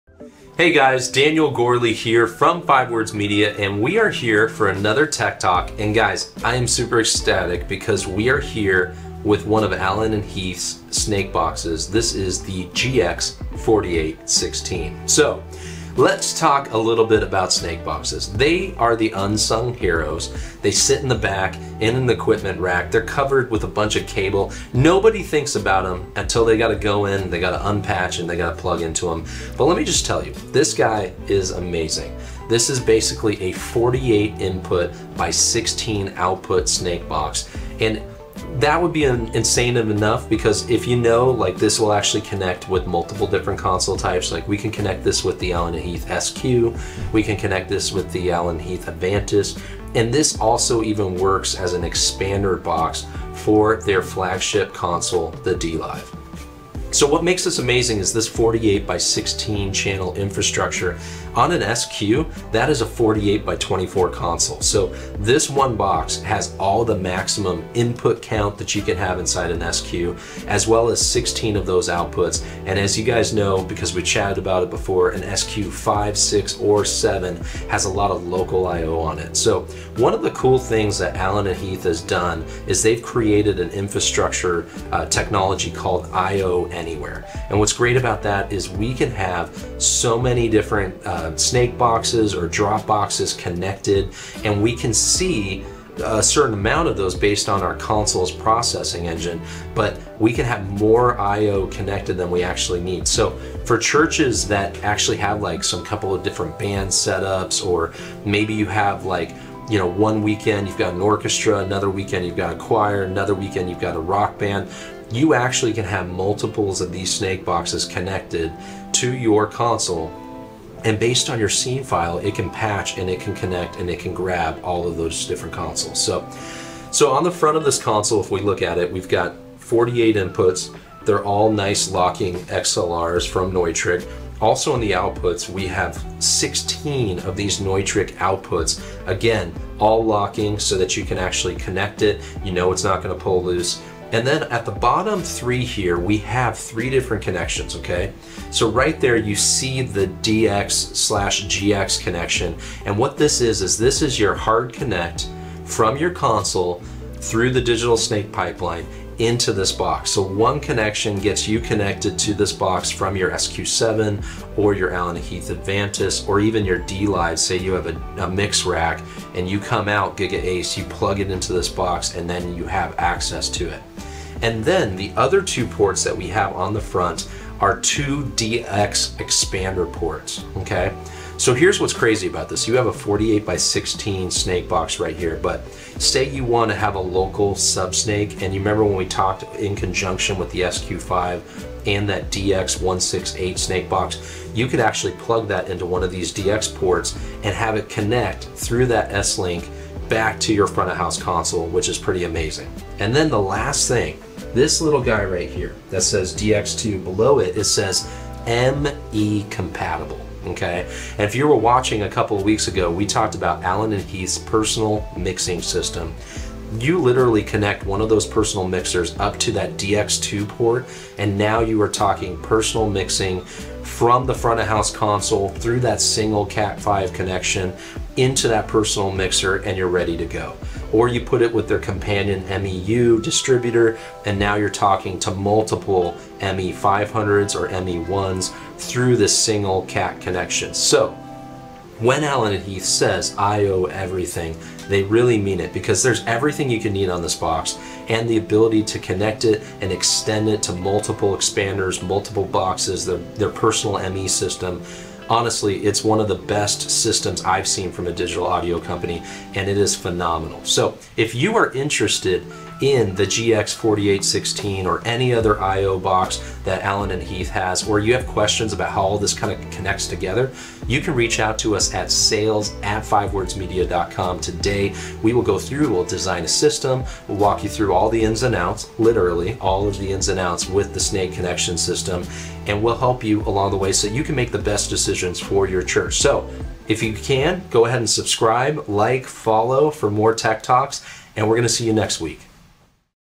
Allen & Heath GX4816 - Tech Talks with Daniel Gourley
We absolutely love these snake box system from Allen & Heath in a multitude of situations. We hope we can provide the info and value you need in the world of AVL.

See. Hear. Feel. Design & Build.